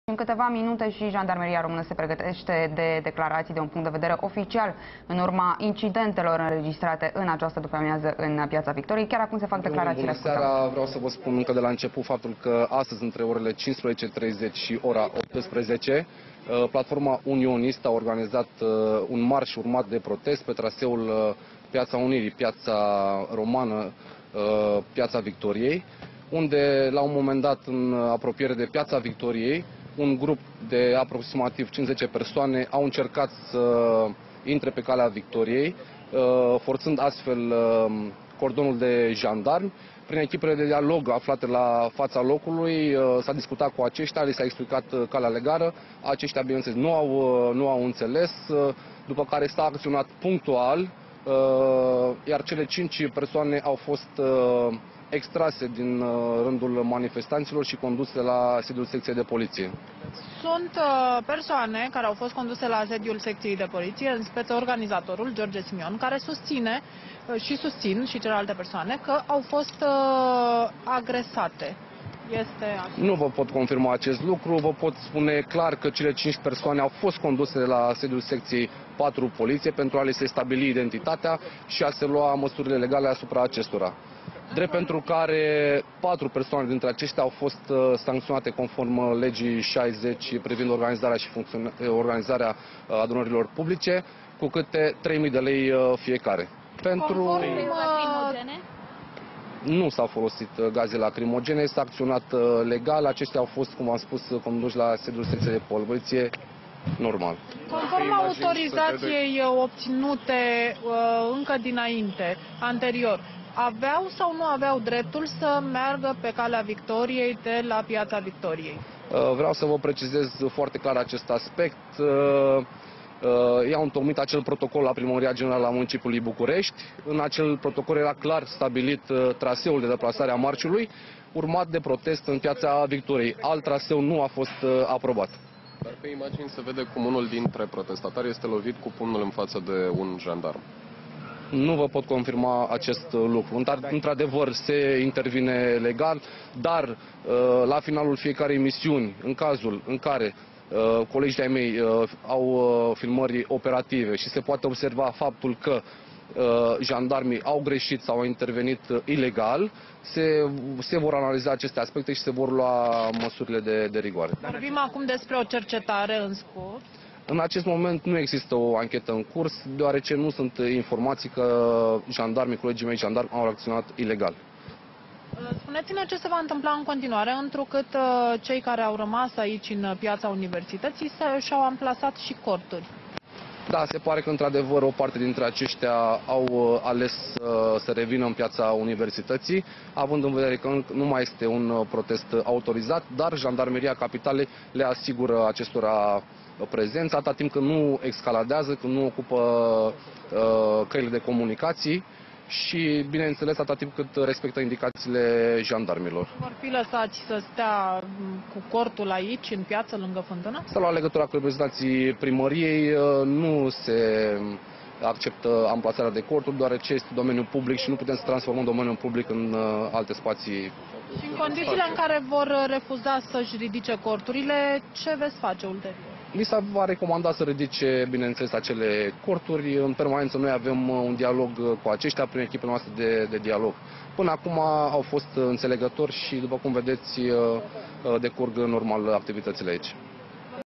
Reacția Jandarmeriei după incidentele de la marșul unionist
Reacția Jandarmeriei după incidentele de la marșul unionist: "În acest moment nu există o anchetă în curs deoarece nu se cunosc acțiuni ilegale"